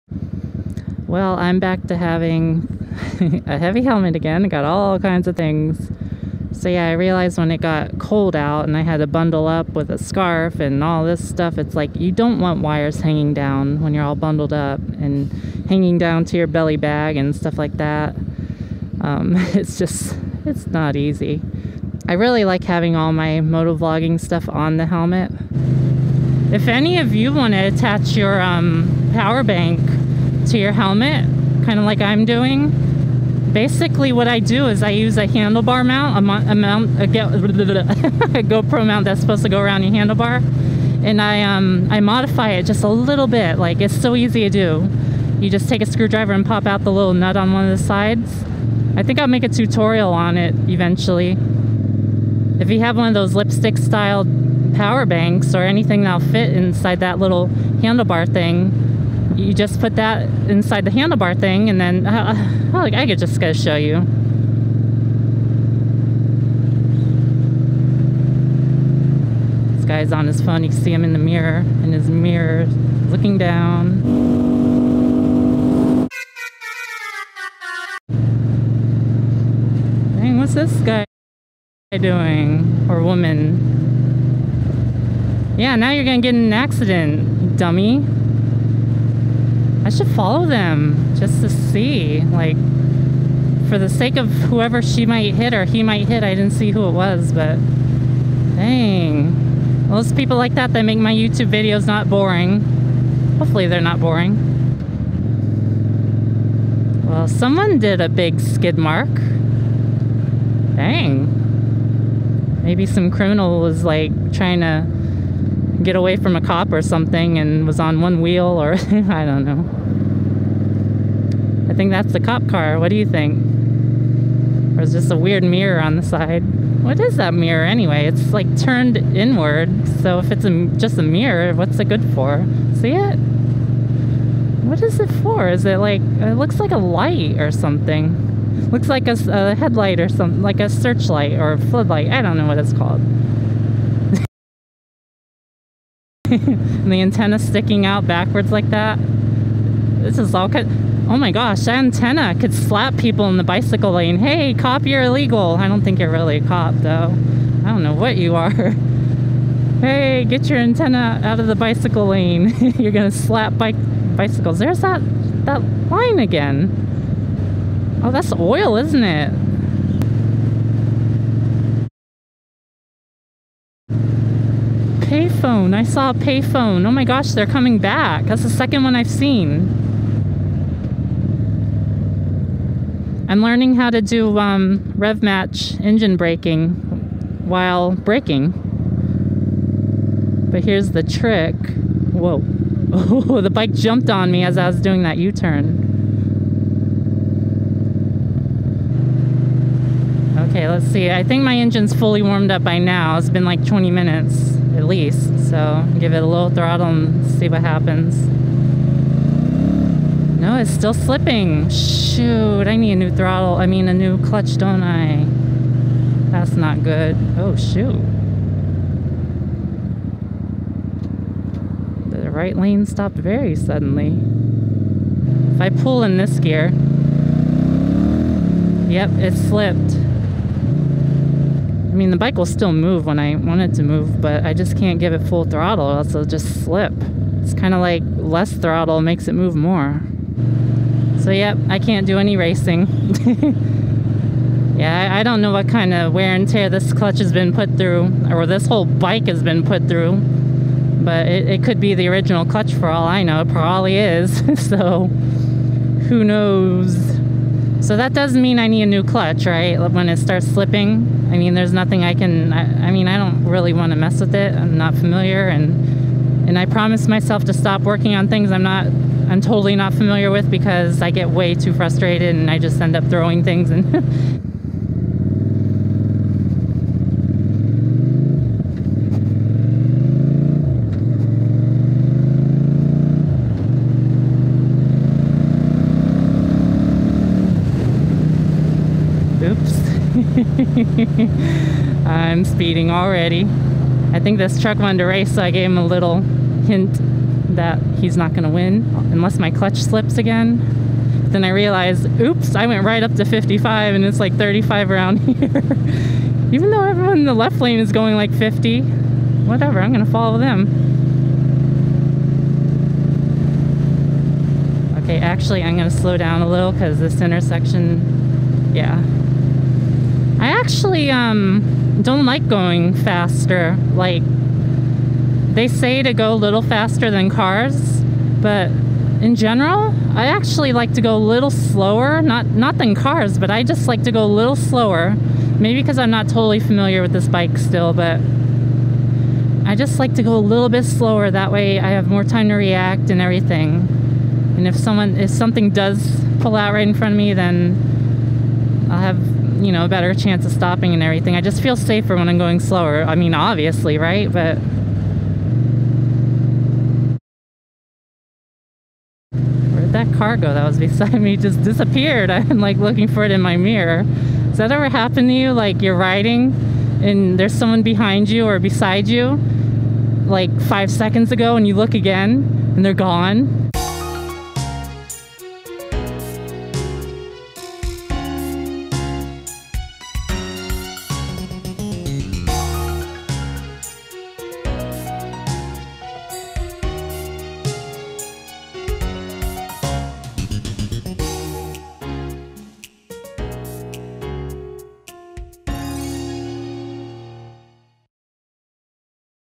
My helmet setup | Clutch slip | Cop car?
I'm always changing my helmet around... here it is currently.